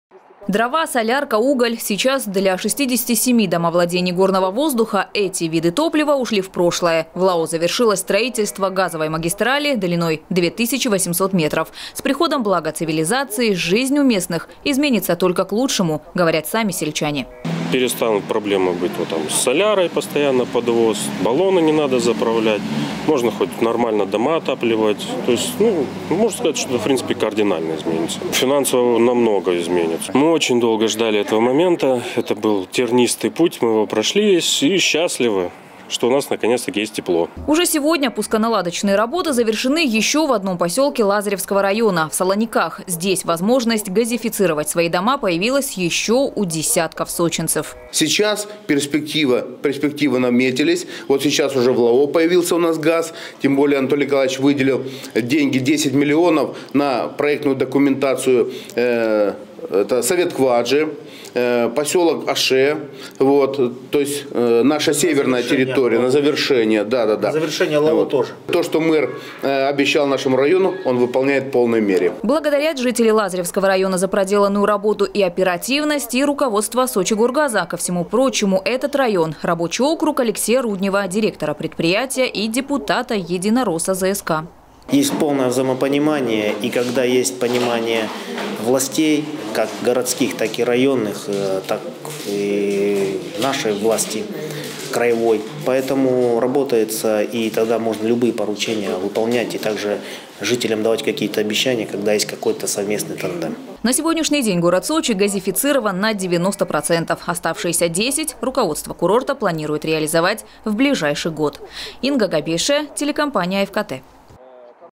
В самом северном районе Сочи построили 2800 метров нового газопровода. Новости Сочи Эфкате
Специалисты Сочигоргаза завершили пуско-наладочные работы в селе Горный воздух.
Сочи. Новости.
Телеканал Эфкате.